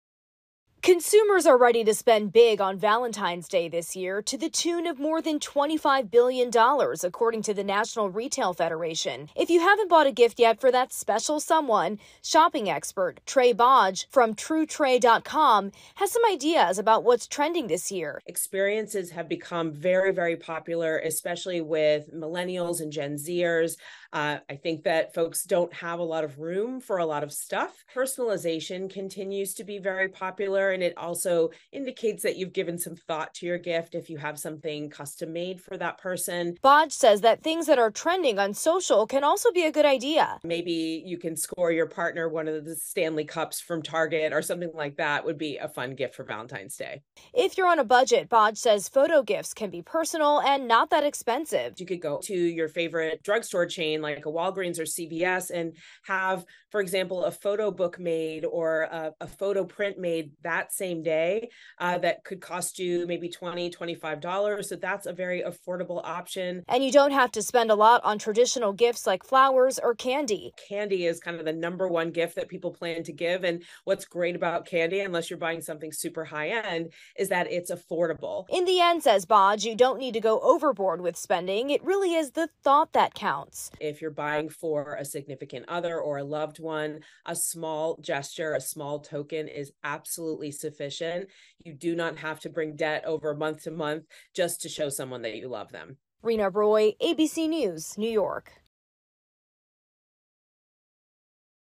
Valentine’s Day trends to keep you from breaking the bank
Consumers are expected to spend big on Valentine’s Day. Experts are sharing ways to help you find the best gifts without breaking the bank.